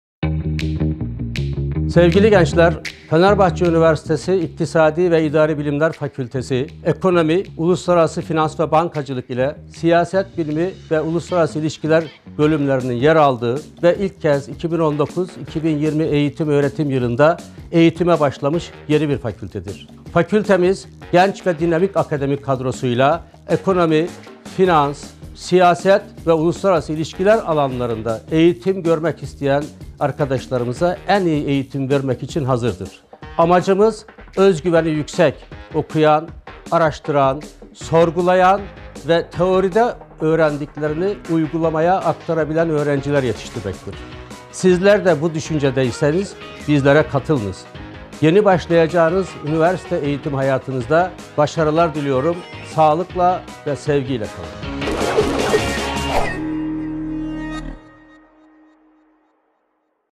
İktisadi ve İdari Bilimler Fakültesi Dekanı Prof. Dr. Şahamet Bülbül'ün Konuşması
İktisadi ve İdari Bilimler Fakültesi Dekanı Prof. Dr. Şahamet Bülbül, amacı; özgüveni yüksek, okuyan, araştıran, teoride öğrendiklerini uygulamaya aktarabilen öğrenciler yetiştirmek olan bölümümüzü tanıtıyor.